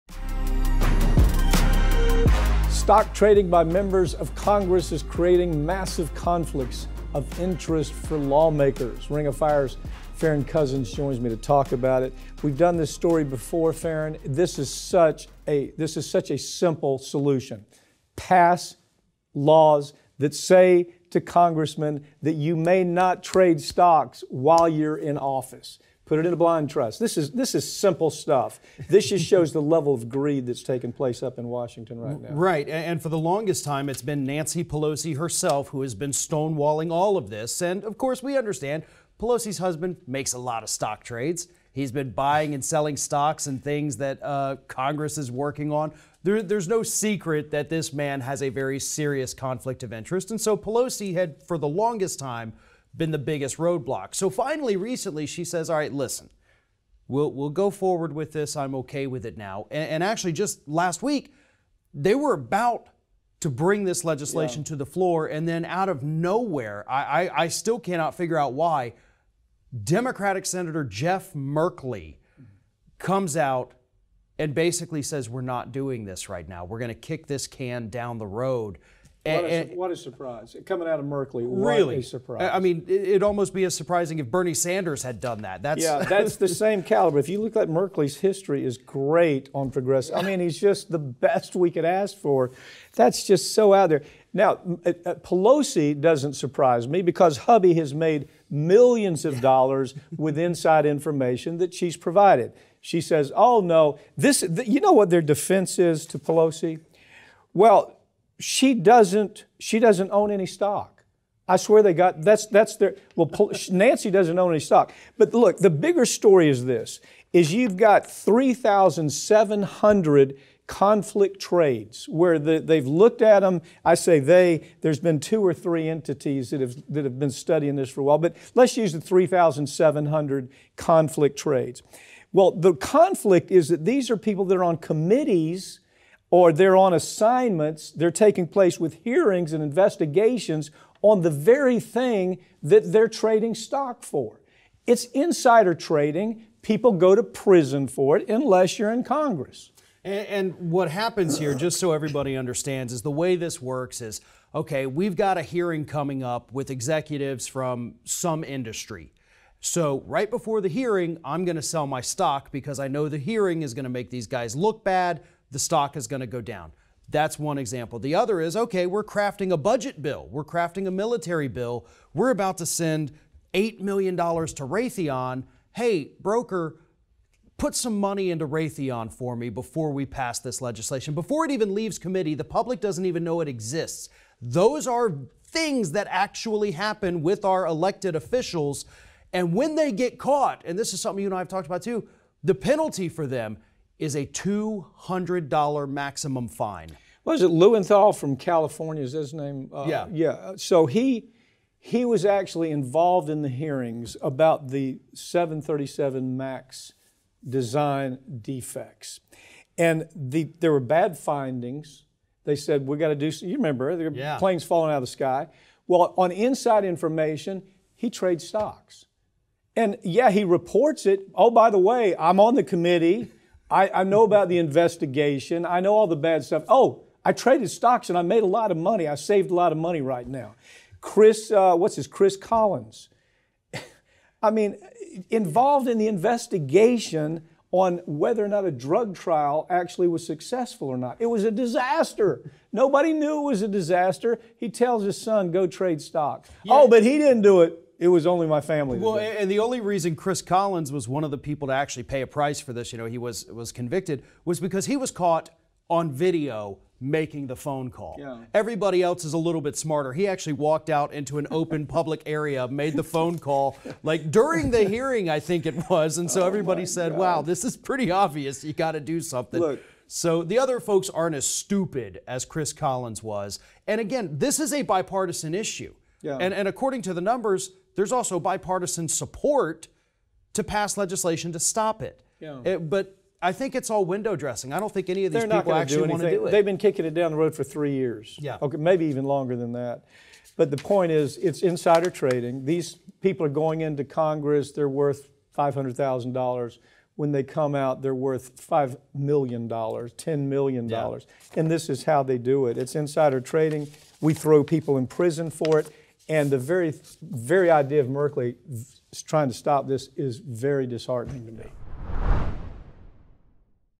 Nearly 20% Of All Lawmakers Engage In Insider Trading
Stock trading by members of Congress is creating massive conflicts of interest for lawmakers. Mike Papantonio & Farron Cousins discuss more.

Stock trading by members of Congress is creating massive conflicts of interest for lawmakers. Ring of Fire's Farron Cousins joins me to talk about it. We've done this story before, Farron. This is such a, this is such a simple solution, pass laws that say to congressmen that you may not trade stocks while you're in office. Put it in a blind trust. This is, this is simple stuff. This just shows the level of greed that's taking place up in Washington right now.
Right. And, and for the longest time it's been Nancy Pelosi herself who has been stonewalling all of this and of course, we understand Pelosi's husband makes a lot of stock trades. He's been buying and selling stocks and things that Congress is working on. There, there's no secret that this man has a very serious conflict of interest. And so Pelosi had for the longest time been the biggest roadblock. So finally, recently she says, all right, listen, we'll, we'll go forward with this. I'm okay with it now. And, and actually just last week, they were about to bring this legislation to the floor and then out of nowhere, I, I, I still cannot figure out why, Democratic Senator Jeff Merkley comes out and basically says, we're not doing this right now. We're gonna kick this can down the road. And.
What, what a surprise coming outta Merkley.
Really.
I mean, it'd almost be as surprising if Bernie Sanders had done that. That's.
Yeah. That's the same caliber. If you look at Merkley's history is great on progressive. I mean, he is just the best we could ask for. That's just so out there. Now, Pelosi doesn't surprise me because hubby has made millions of dollars with inside information that she's provided. She says, oh no. This, you know what their defense is to Pelosi? Well, she doesn't, she doesn't own any stock. I swear to God, that's, that's their, well Nancy doesn't own any stock. But look, the bigger story is this is you've got 3,700 conflict trades where they've looked at 'em. I say they, there's been two or three entities that have, that have been studying this for a while, but let's use the 3,700 conflict trades. Well, the conflict is that these are people that are on committees or they're on assignments, they're taking place with hearings and investigations on the very thing that they're trading stock for. It's insider trading. People go to prison for it unless you're in Congress.
And, and what happens here, just so everybody understands is the way this works is, okay, we've got a hearing coming up with executives from some industry. So right before the hearing, I'm gonna sell my stock because I know the hearing is gonna make these guys look bad. The stock is gonna go down. That's one example. The other is okay, we're crafting a budget bill. We're crafting a military bill. We're about to send $8 million to Raytheon. Hey, broker, put some money into Raytheon for me before we pass this legislation, before it even leaves committee, the public doesn't even know it exists. Those are things that actually happen with our elected officials and when they get caught, and this is something you and I have talked about too, the penalty for them is a $200 maximum fine.
Was it Lowenthal from California, is that his name?'.
Yeah.
Yeah. So he, he was actually involved in the hearings about the 737 max design defects and the, there were bad findings. They said, we gotta do, you remember it.
Yeah.
There planes falling outta the sky. Well, on inside information, he trade stocks and yeah, he reports it. Oh, by the way, I'm on the committee. I, I know about the investigation. I know all the bad stuff. Oh, I traded stocks and I made a lot of money. I saved a lot of money right now. Chris, what's his, Chris Collins. I mean, involved in the investigation on whether or not a drug trial actually was.